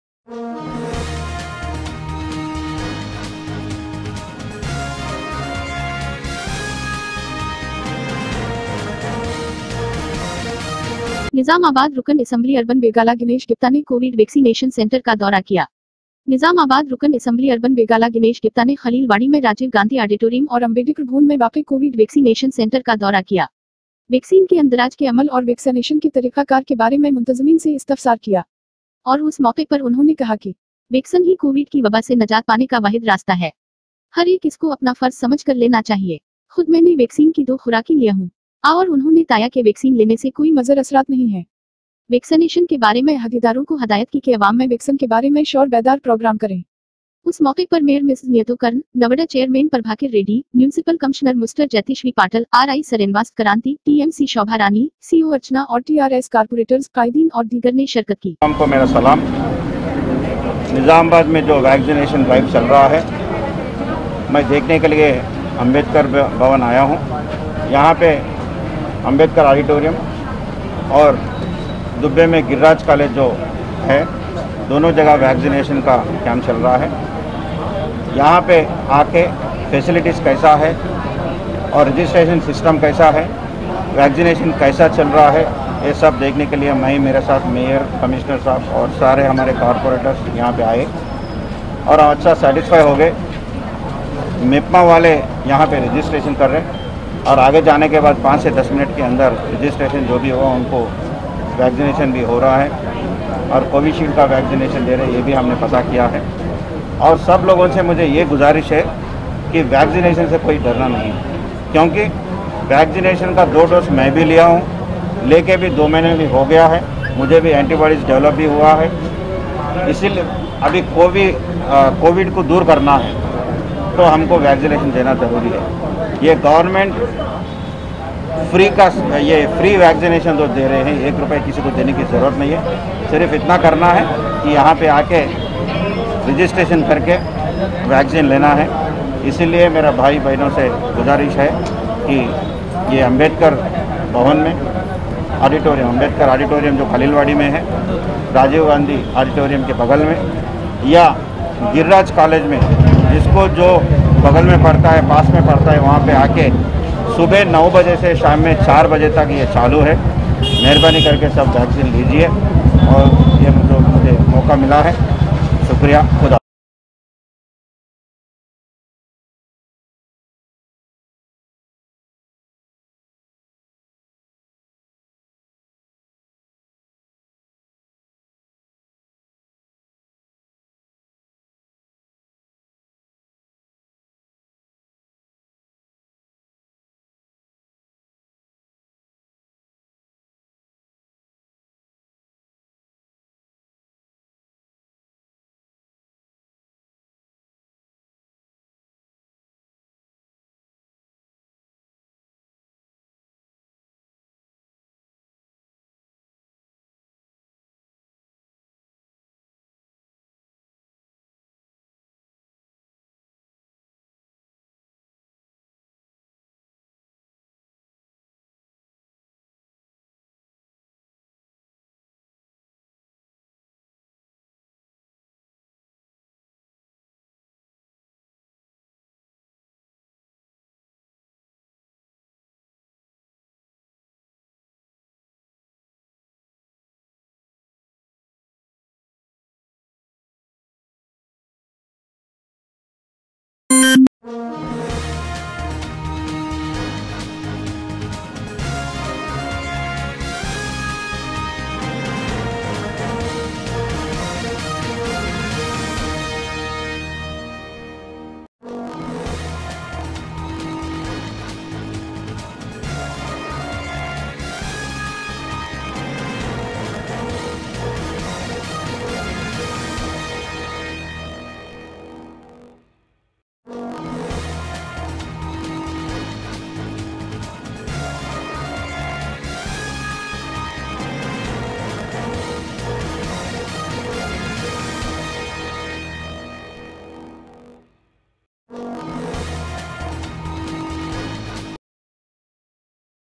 Bigala Ganesh Grupta MLA Nizamabad Urban Visited Covid 19 Vaccination Centers
(TABSERA NEWS)

ganesh gupta
visited centwrs